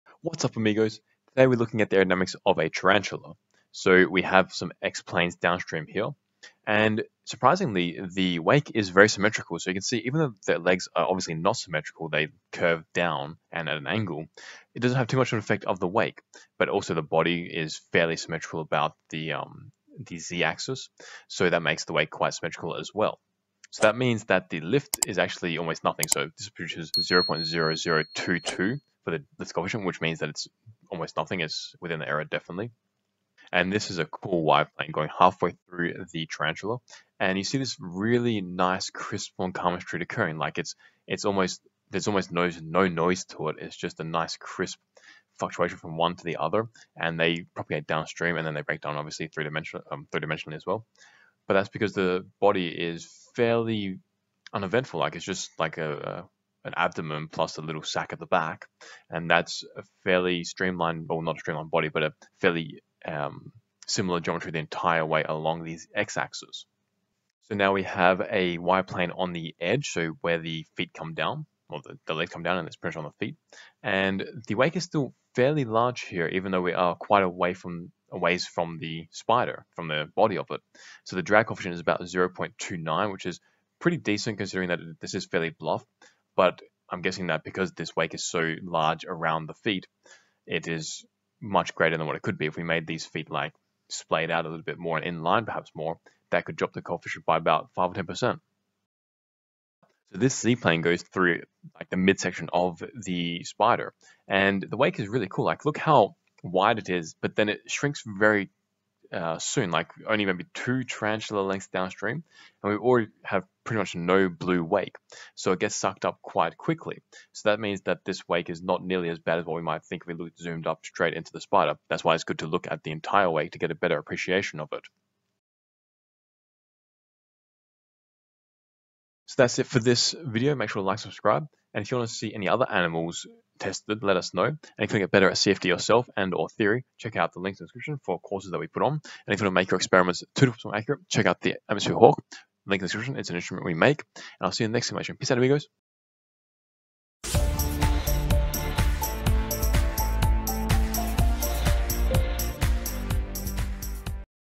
Are tarantulas aerodynamic?
Find out more in this CFD simulation with the Lattice Boltzmann Method!

Make your experiments easier by measuring the density of air with our Atmosphere Hawk